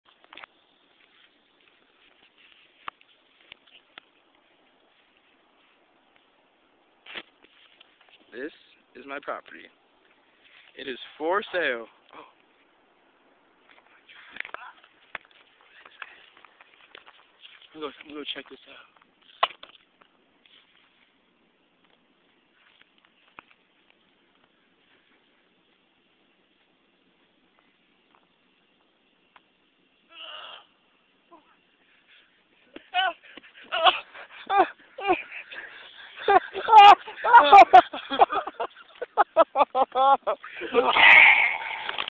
Collin the Killer
In  the deep, deep woods of Macedonia Ohio lives a twisted man named Collin the killer. Here is some very gruesome footage of this sick minded man